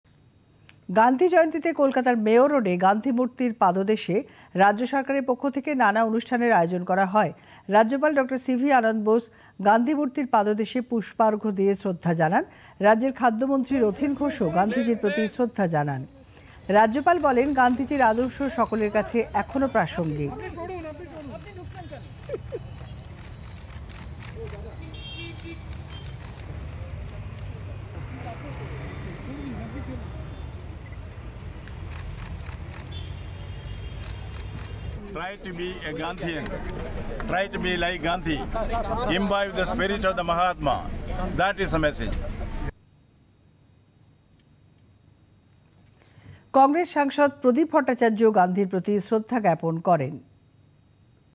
গান্ধী জয়ন্তী উপলক্ষ্যে মেয়ো রোডে অনুষ্ঠান
কলকাতার মেয়ো রোডে গান্ধী মূর্তির পাদদেশে পুষ্পার্ঘ্য দিয়ে শ্রদ্ধা জানান রাজ্যপাল ডঃ সি ভি আনন্দ বোস।